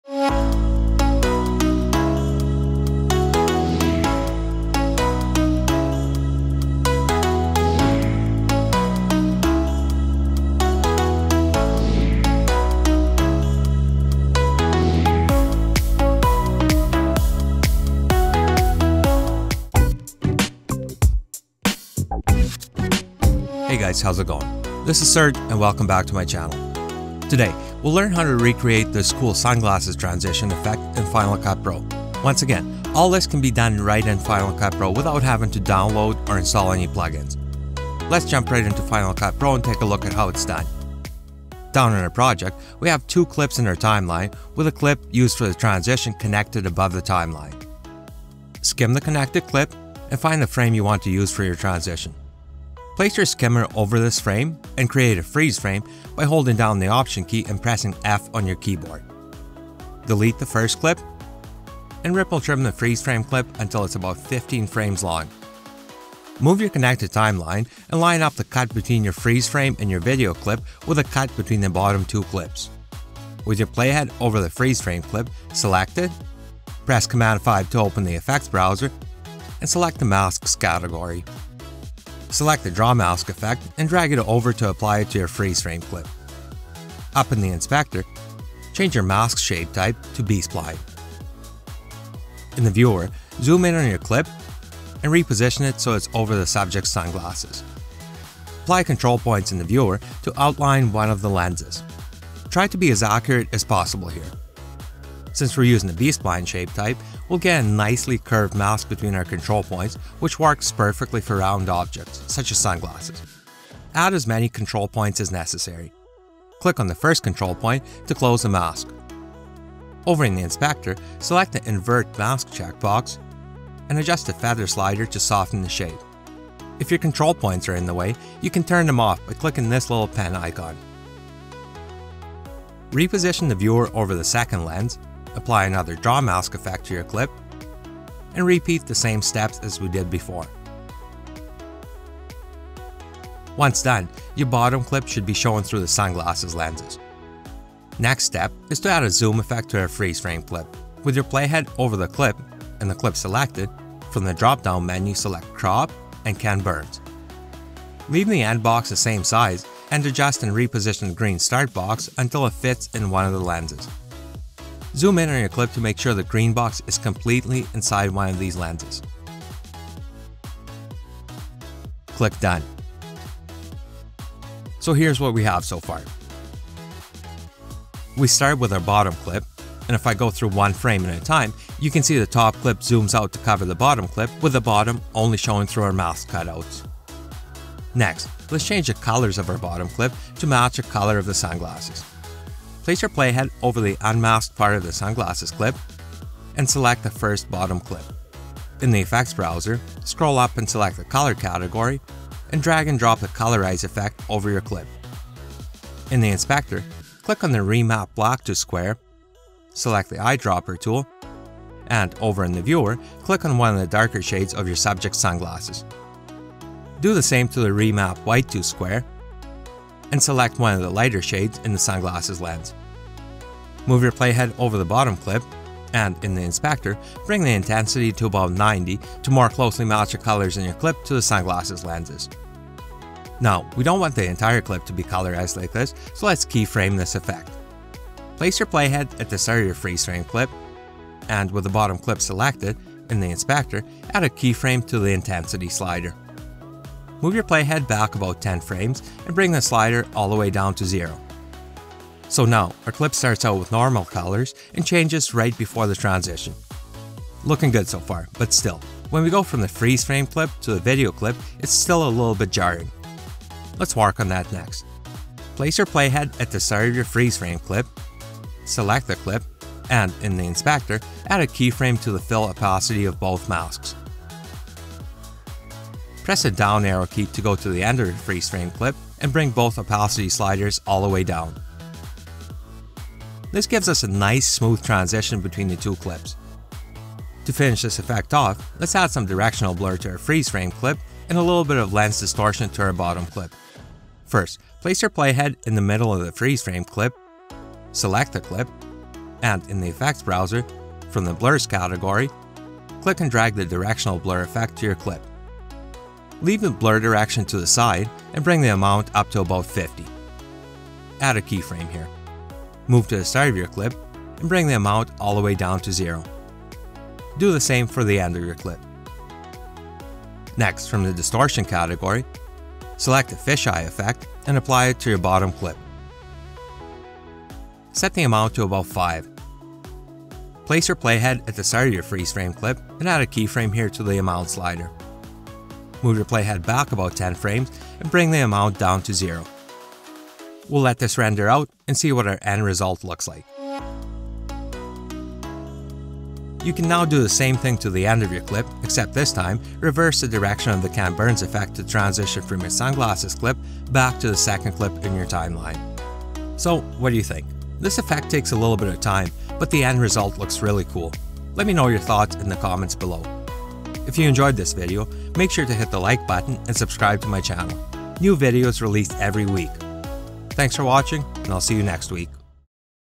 Awesome Sunglasses Transition - Final Cut Pro Tutorial 2019
In this video, you will learn how to create an awesome sunglasses reflection transition in Final Cut Pro, without the need to download or install any plugins. This transition is achieved by using a combination of the Ken Burns Effect, Mask Tool, Colorize Effect and the Fisheye Effect, for an awesome looking video transition.